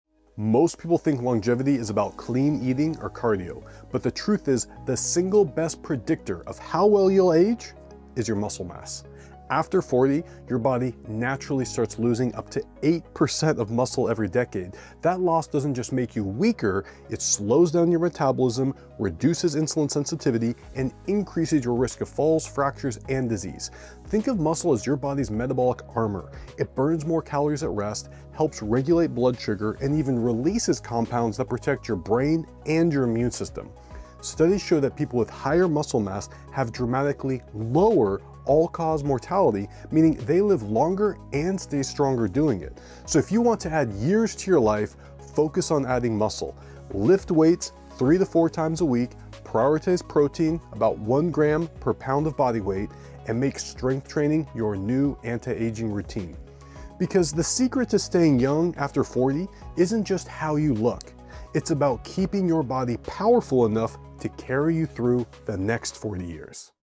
The #1 Predictor of Aging Well: Muscle Mass
Most people think longevity is all about clean eating or cardio… but the single best predictor of how well you’ll age is your muscle mass.

After 40, we naturally lose up to 8% of muscle every decade.
And that loss doesn’t just make you weaker it slows your metabolism, reduces insulin sensitivity, and increases the risk of falls, fractures, and chronic disease.

Think of muscle as your body’s metabolic armor:
💪 Burns more calories at rest
⚡ Stabilizes blood sugar
🧠 Releases compounds that protect your brain
🛡️ Supports your immune system

Research shows people with higher muscle mass have dramatically lower all cause mortality. In other words they live longer and stay stronger.

If you want to add years to your life, focus on adding muscle:
✔️ Lift weights 3–4 times a week
✔️ Prioritize protein (~1g per lb of body weight)
✔️ Make strength training your new anti-aging routine

The secret to staying young after 40 isn’t how you look it’s keeping your body powerful enough to carry you through the next 40 years.

Send us a question you want to be answered in a future video through our quick & easy Google Form